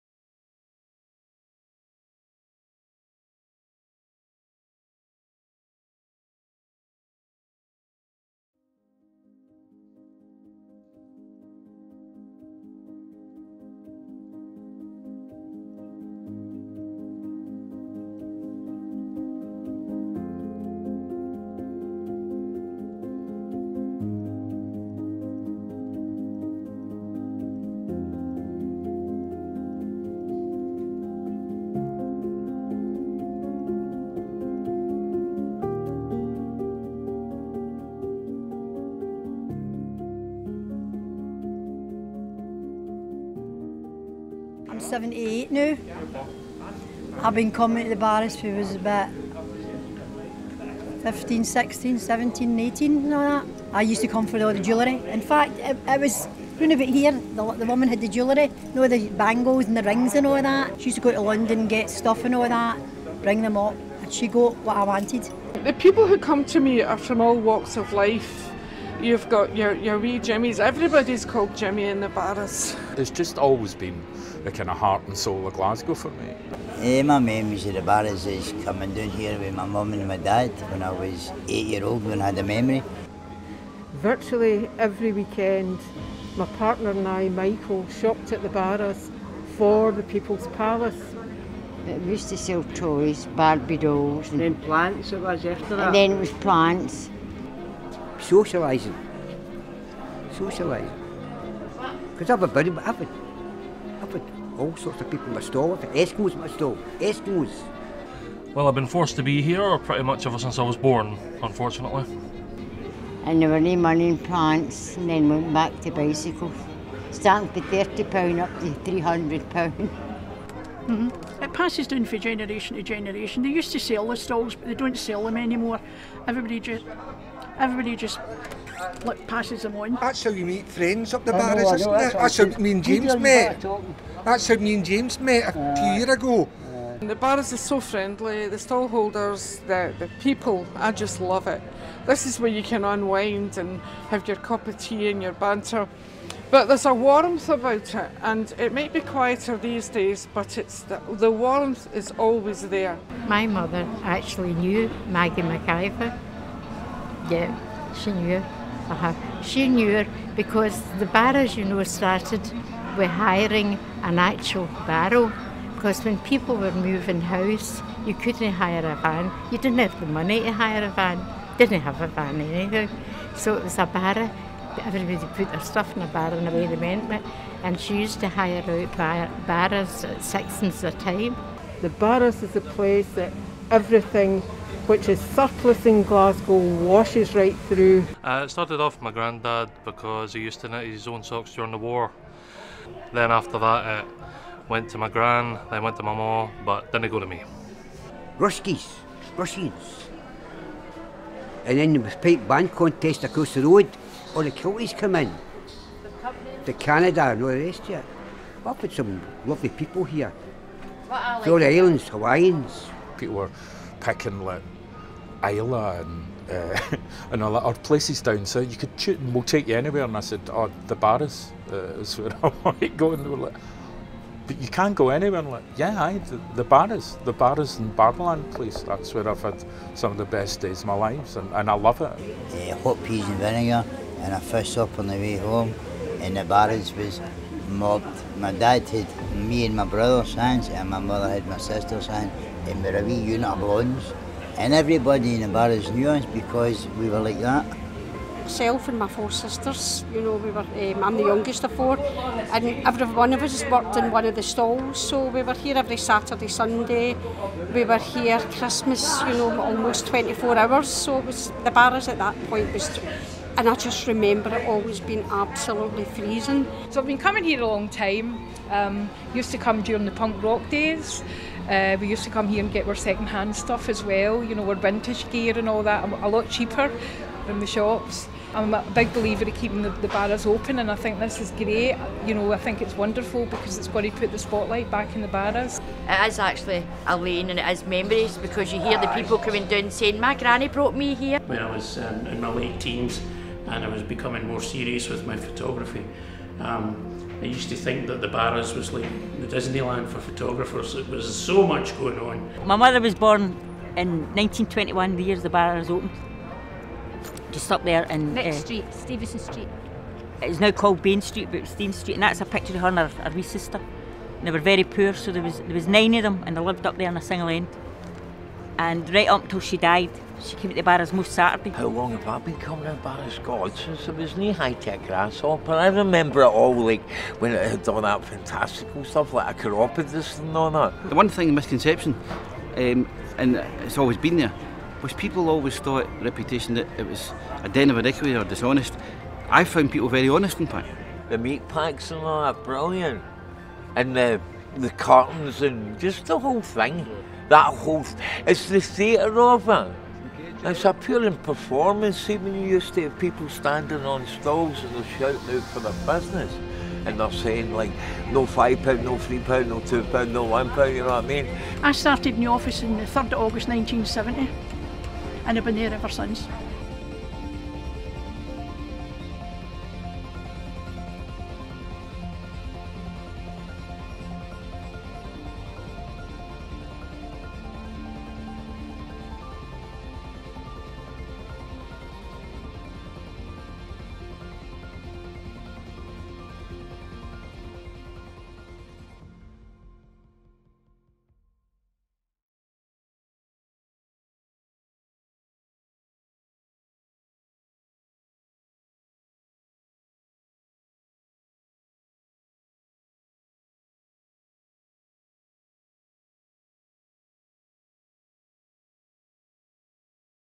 Barras100 - Capture the Moment - A Short Documentary
This short was filmed, directed and edited by Sophie Mckenzie, Niamh Breslin and Wiktoria Matyiak, while working with the Barras100 project.

With support from:

Bash Khan - editing and workshop facilitating
Cass Mcgrogan - interview filming
Molly Mckeagney - interview filming
Friends of The Pipe Factory - production

Barras100 was supported by Creative Scotland, Glasgow City Heritage Trust, Scotland Loves Local and Glasgow City Council.